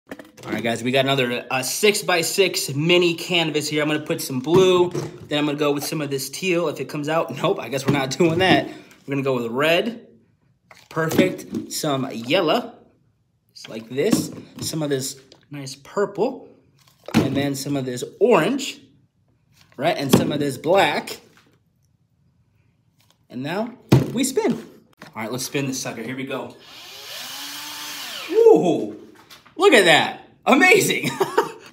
Spin Art On A MINI Canvas!
Spin Art On A MINI Canvas! This small black 6x6 canvas turned out better than I thought! Let me know what you guys think in the comments below!

Johnny Q
511 E Walnut Street